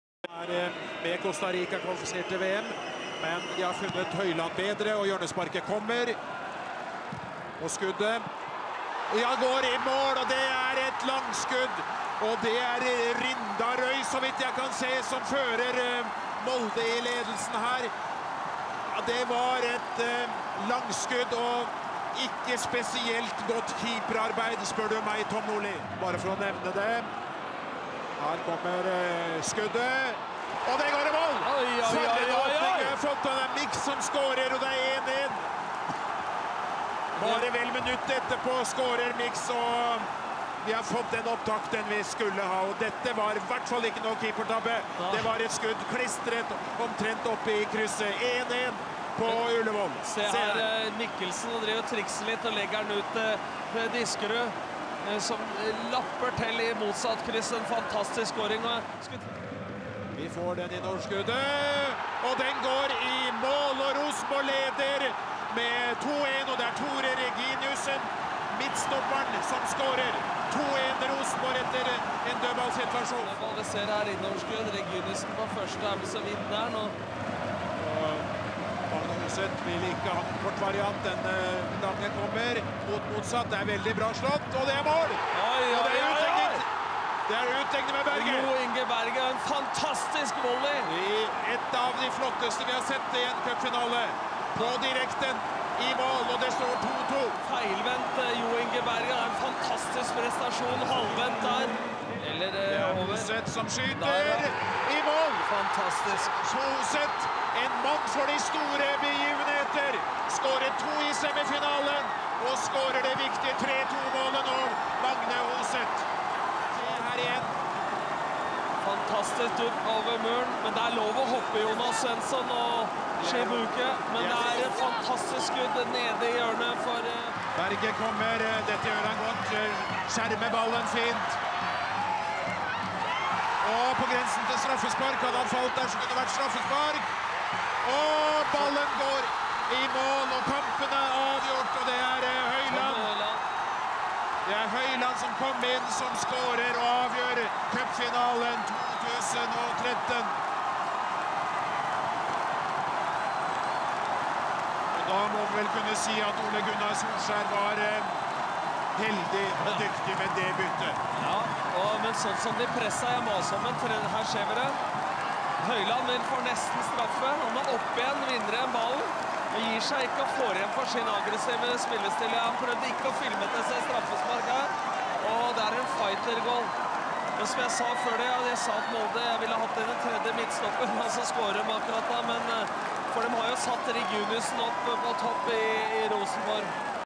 Cupfinale 2013 Molde-Rosenborg 4-2
Cupfinalen mellom Molde og Rosenborg 24.11.13

Målscorere:
MFK:(Rindarøy 15", Berget 71", Hoseth 81", Høiland 89")
RBK:(Diskerud 17", Reginiussen 48")